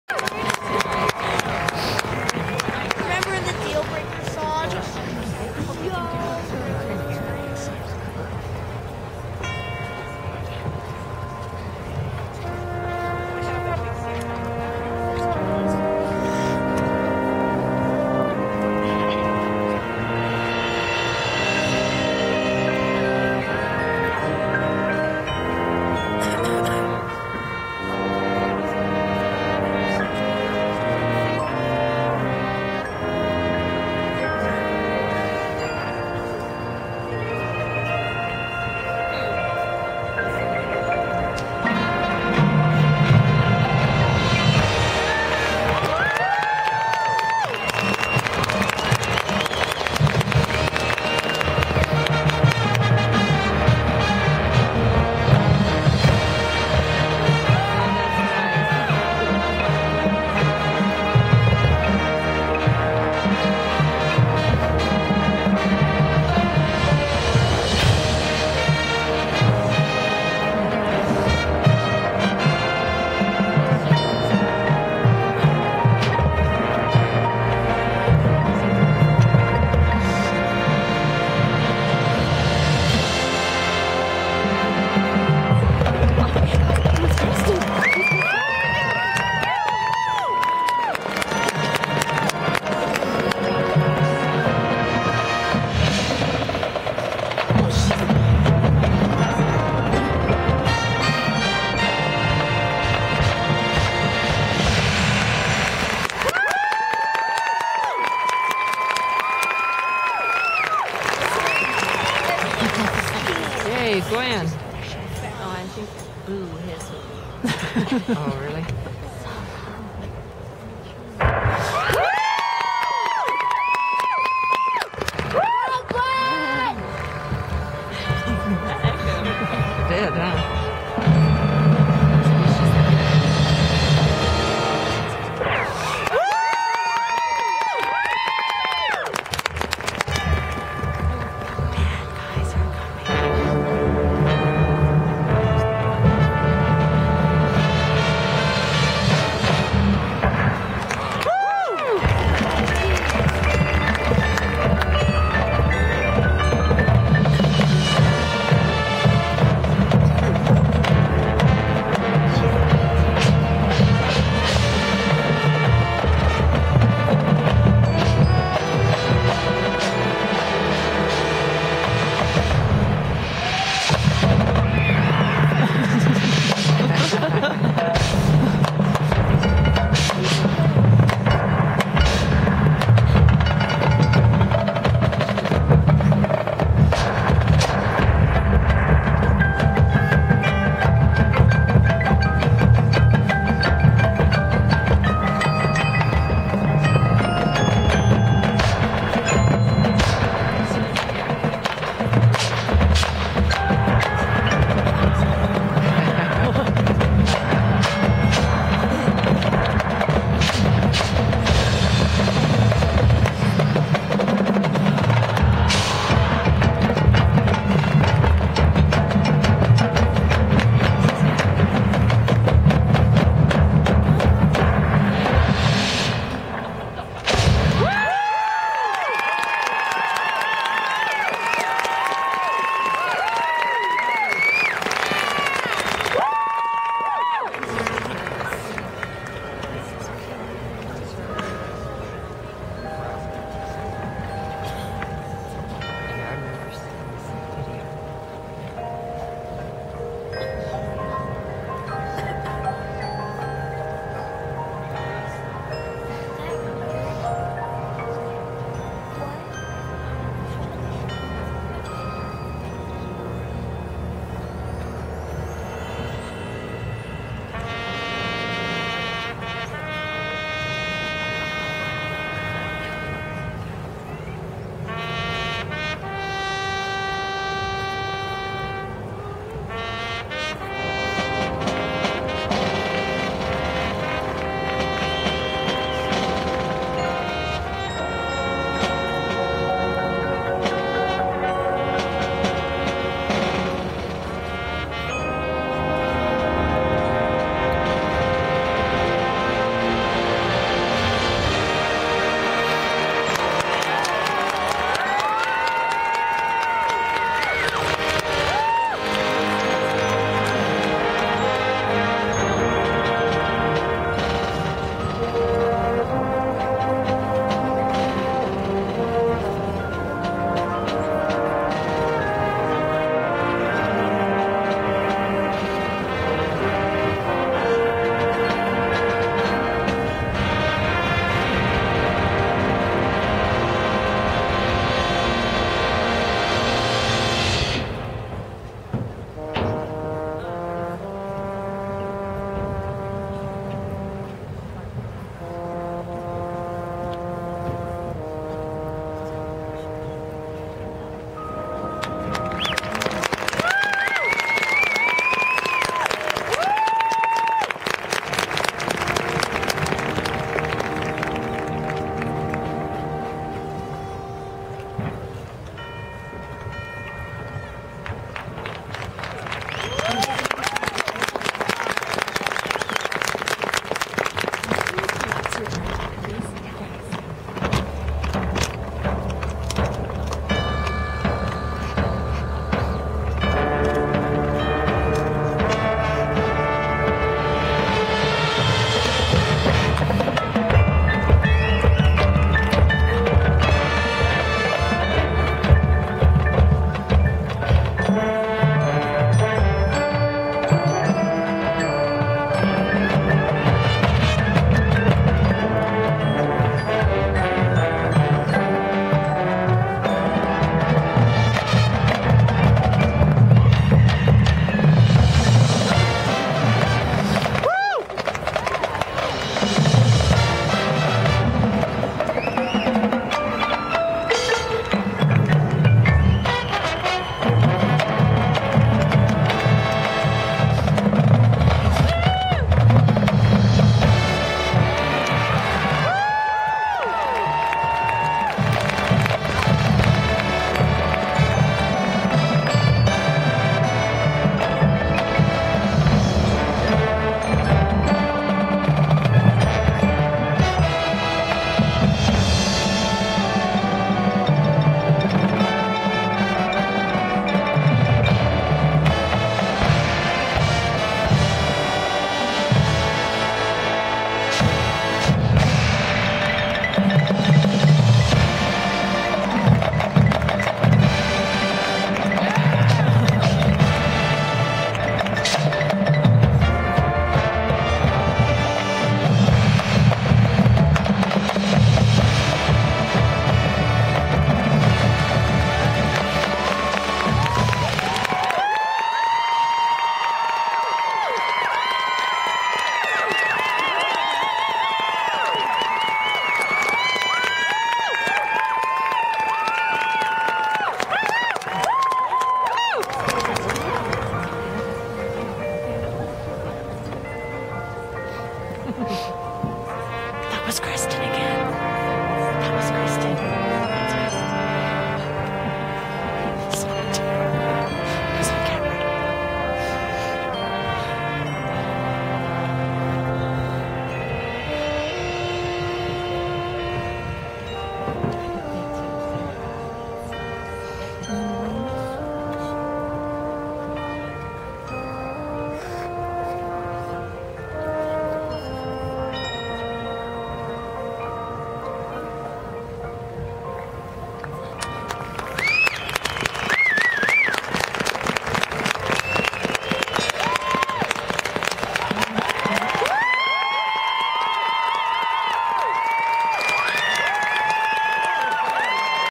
Westlake Utah Marching Band -Rocky Mountain Invitational @BYU 2013 2nd place
Awarded Outstanding Visual Performance (with a near perfect score) Outstanding Color Guard and 2nd place overall!
Fantastic Performance -so-so recording.